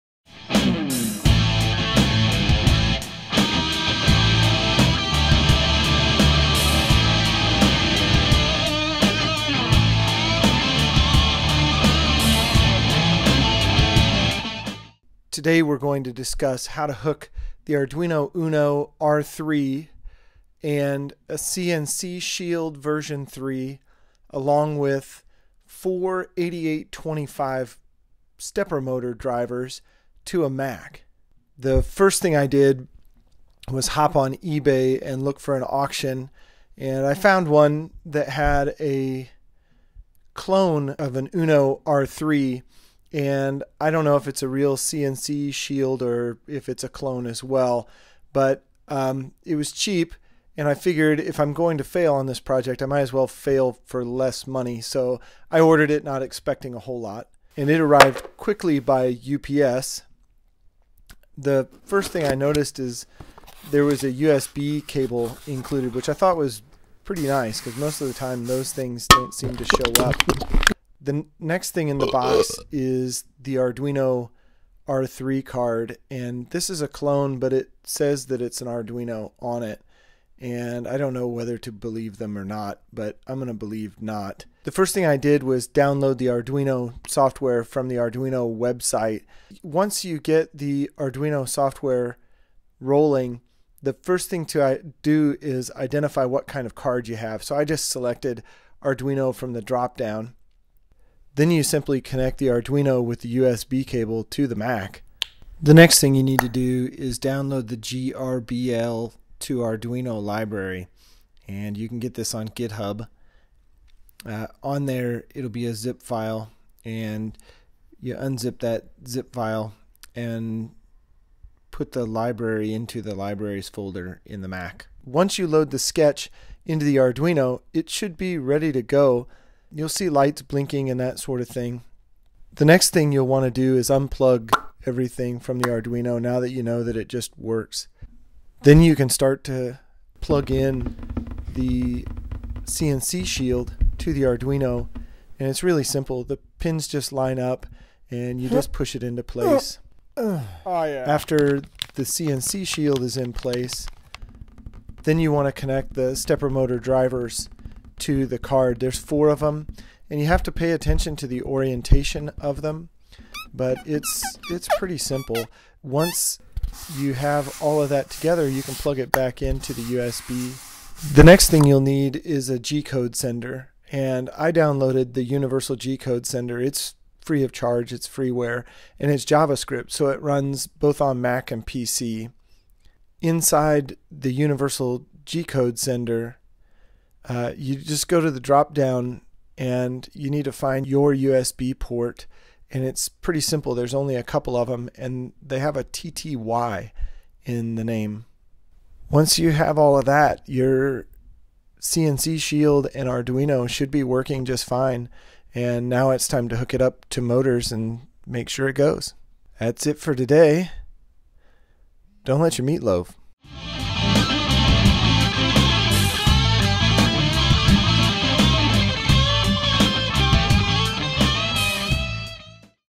Arduino + CNC + Mac - DIY CNC Controller Made Easy
Some fun stop motion animation showing how to get your Arduino Uno R3, CNC Shield V3.00 & 8825 motor drivers up and running on a Mac.

1. Install the Arduino App
2. Unzip the GRBL Library to your Arduino Library folder (rename it to GRBLArduino first)
3. Plug your Arduino into your Mac's USB port.
4. Open the Arduino App, Goto "Tools/Board" and select your model of Arduino from the list
5. Open the sketch "GRBL to Arduino", then click "upload" it to install it on your Arduino.
6. Unplug your Arduino from your computer.
7. Install the CNC Shield, the (4) 8825 motor controllers & the heat sinks if you have them.
8. Wire a suitable power supply and your stepper motors to the CNC Shield
8. Plug the completed assembly into your Mac.
9. Start the Universal G-Code Sender.JAR Application. *NOTE*: You may need to create a "/var/lock" directory on OSX to fix a bug in the serial library. To do this open the Terminal application and run the following two commands:
sudo mkdir /var/lock
sudo chmod 777 /var/lock
10. Select the USB connector you are using in the "Port" drop down menu.
11. Start sending G-Code and control commands to the Arduino!!!